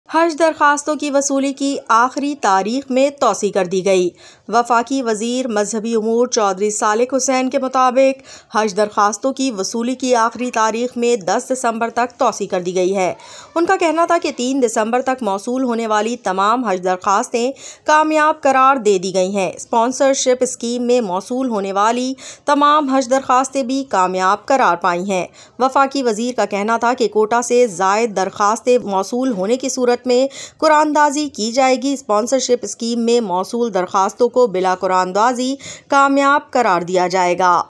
Deadline for Hajj Applications Extended: Latest Update"| SuchExpressNews Official
The Ministry of Religious Affairs has announced an extension for the submission of Hajj applications, providing aspiring pilgrims with additional time to complete their documentation. Stay updated with the new deadlines and requirements for this year's Hajj arrangements. Don't miss this important update!